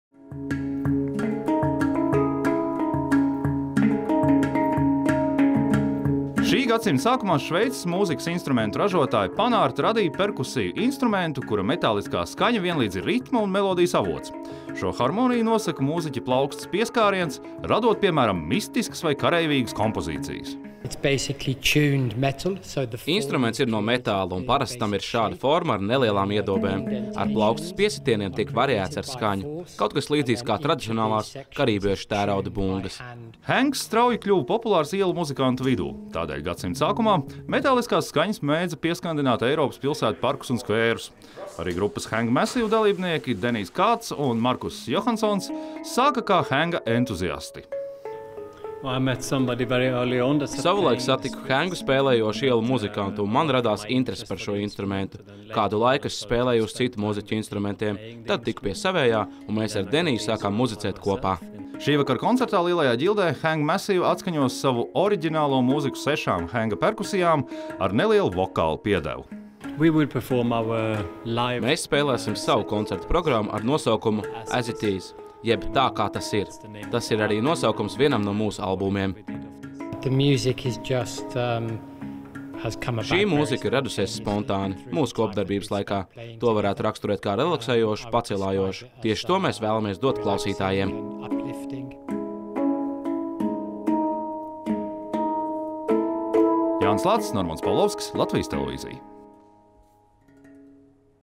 Rīgā muzicē “Hang Massive”
Vai jūs zināt, kas ir hengs? Šī gadsimta sākumā radīts instruments, kas strauji kļūst populārs eksperimentālās mūzikas cienītāju vidū! Latvijā šo mūzikas instrumentu spēlē Raimonds Tiguls, bet pasaulē par henga spēles virtuoziem uzskata duetu “Hang Massive”. Sākotnēji grupa muzicēja Eiropas pilsētu skvēros, bet tagad to aicina arī uz lielām skatuvēm. Šovakar viņi spēlēs Lielajā ģildē, turklāt divus koncertus, jo lielās intereses dēļ tika izziņots papildkoncerts.